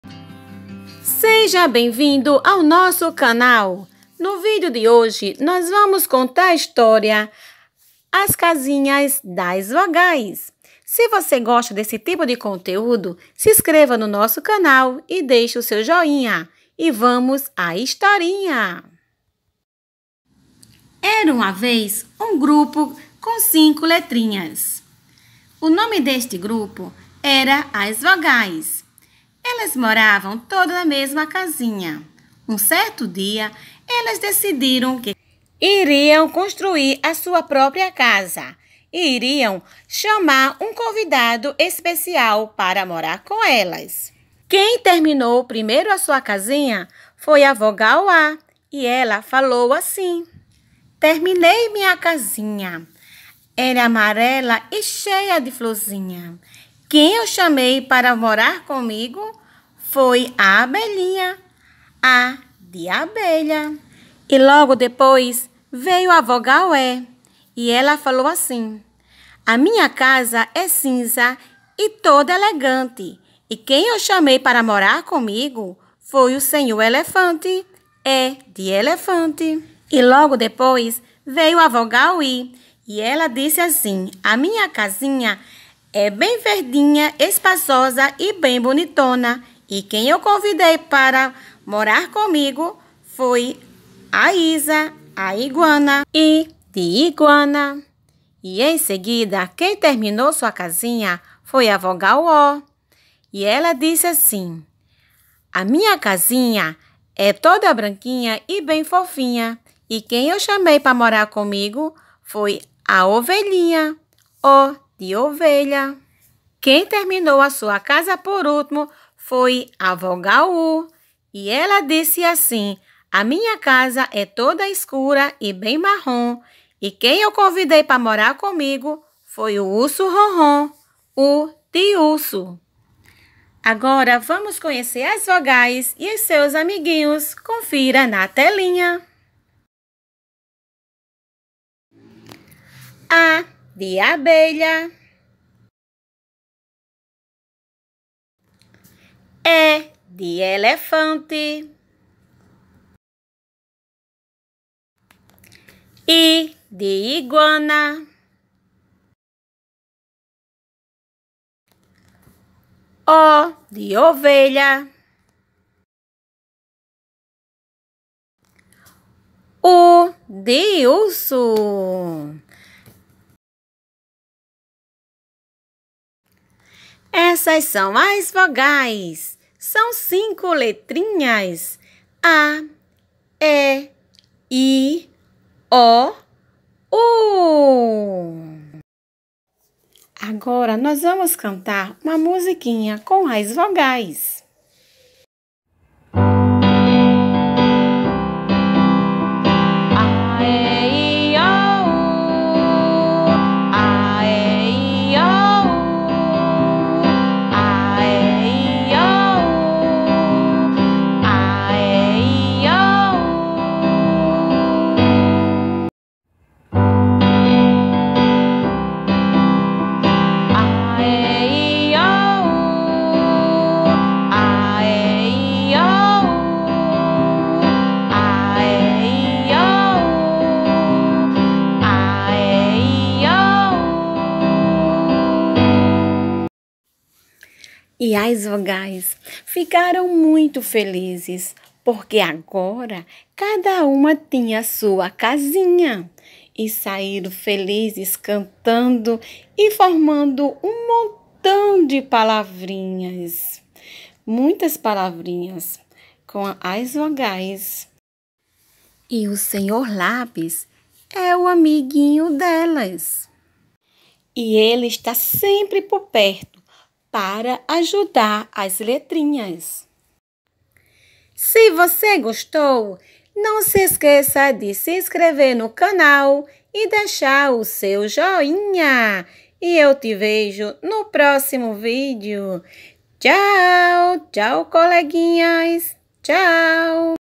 História:A casinha das vogais
asvogais vídeoeducativo História: As casinhas das vogais. Vídeo educativo para Educação Infantil, contando história com as vogais uma forma lúdica de ensinar as vogais para a educação infantil.